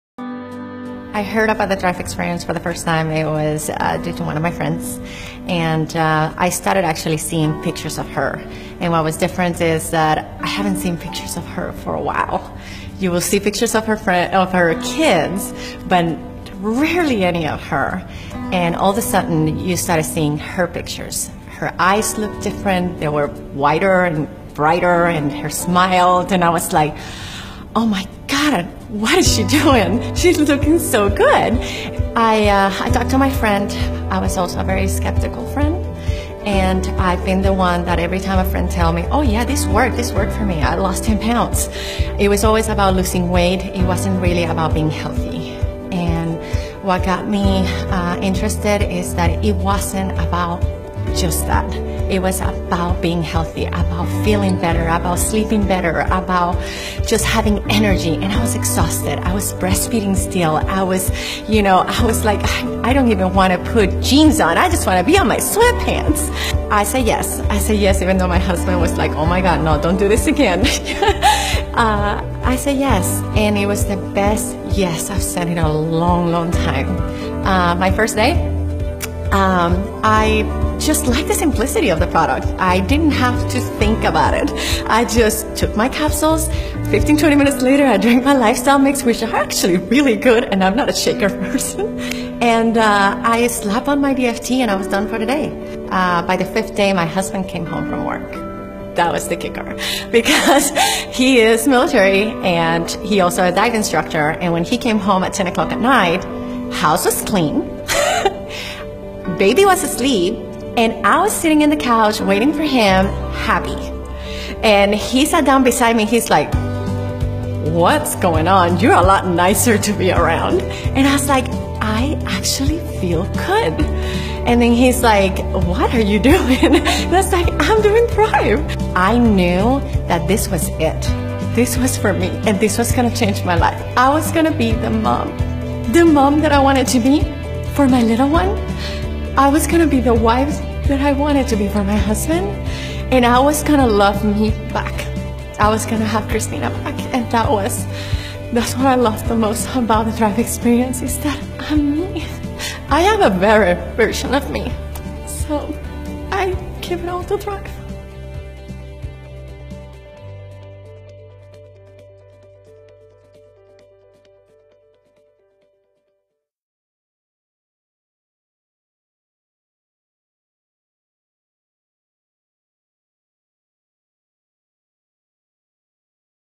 Breastfeeding mom was exhausted - REAL Thrive Experience REVIEW
I want to help you live the life you deserve!

Elise Lininger
200K Multi-Million VIP Top Network Marketing Leader